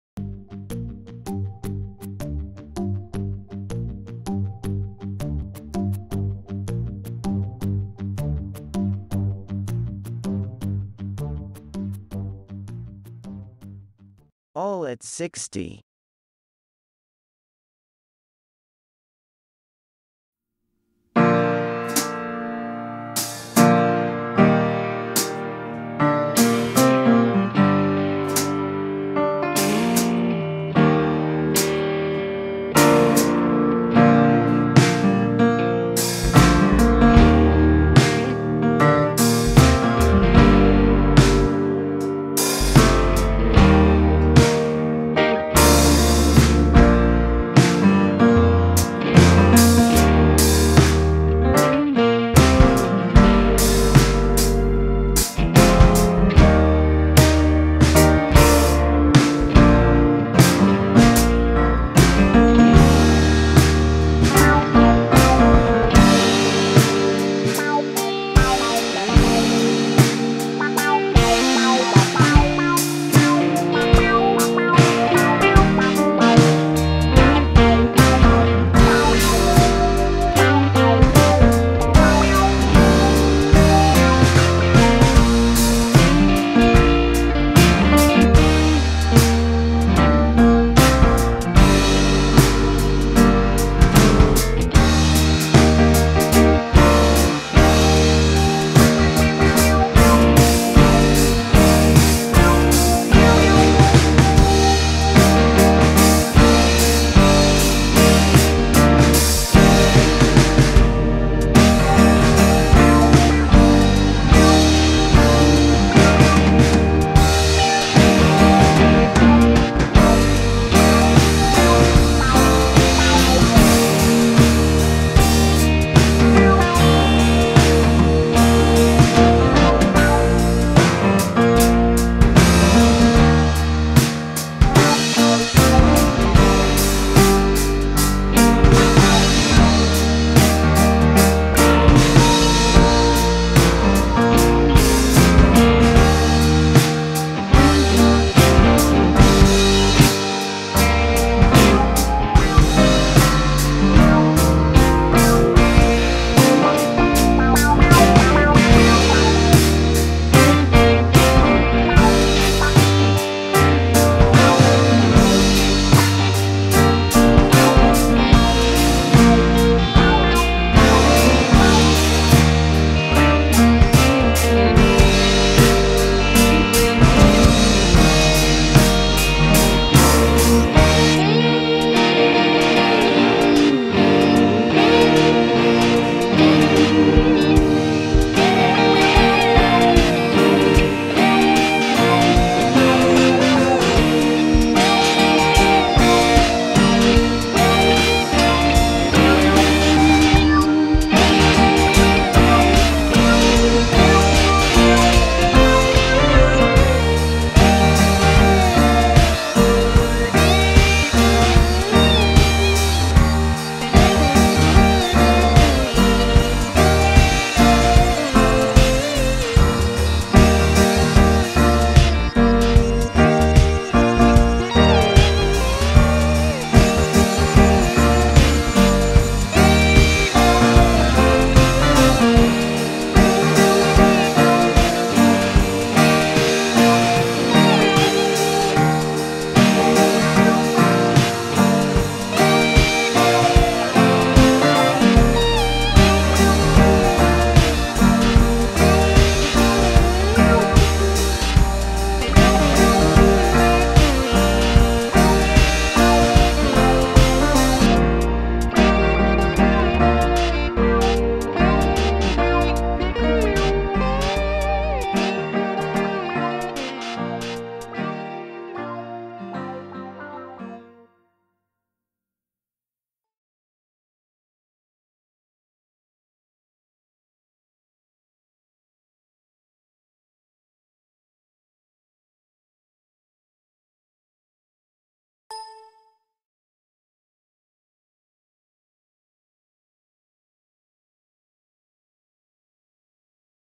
HOW TO MAKE MOVING BACKGROUND IN POWERPOINT by: ALL AT SIXTY
This tutorial shows how to make moving background in powerpoint using simple animation.